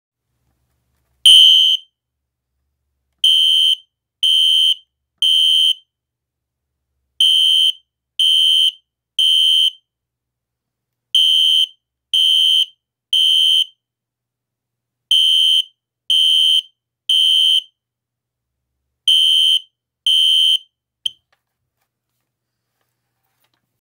System Sensor SpectrAlert Classic H1224W Test
Correction: it was on High Volume the entire time, and now I put it on low volume.